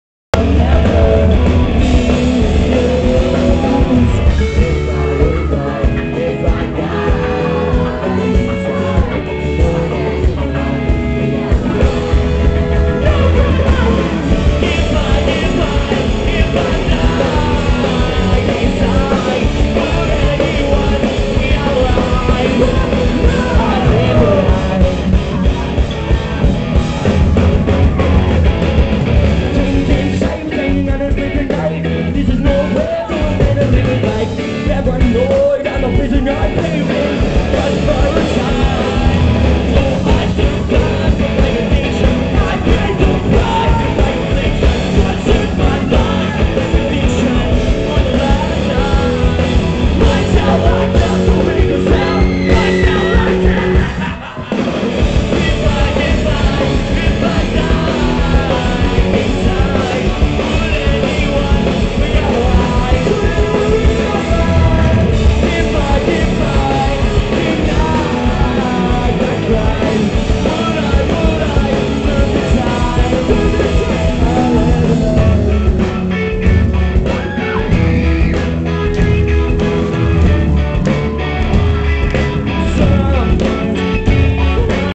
cute is what we aim for @ The Magic Stick Detroit
cute is what we aim for at the magic stick in detroit michigan on August 16, 2008.